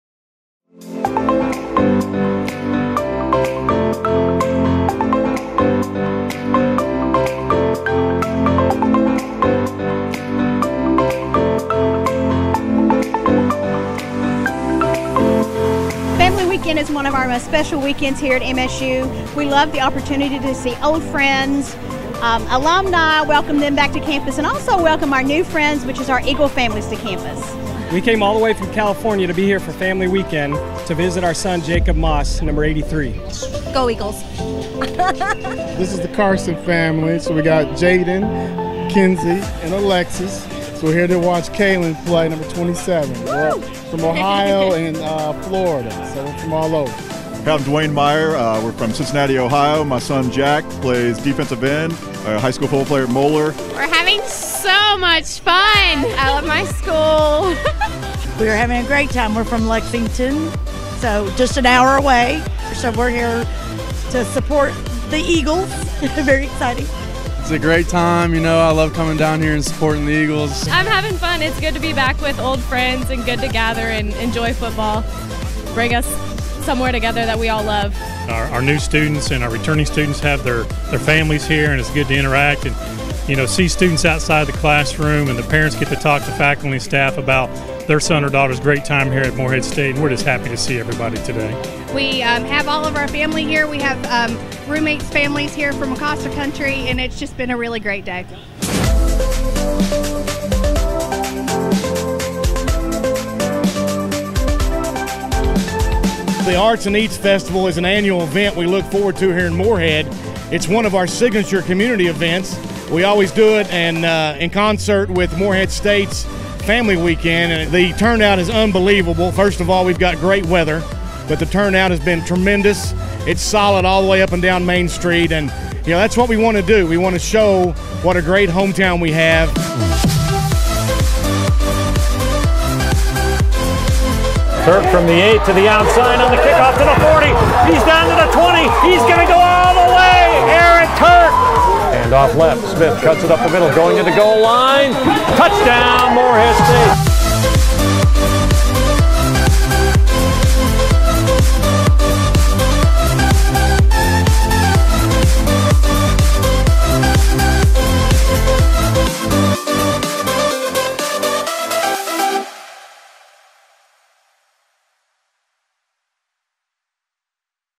Morehead State Family Weekend Recap 2018
Family and friends of Morehead State students got a taste of life in the Eagle Nation this weekend during the 40th Annual Family Weekend, Sept. 14-16, 2018.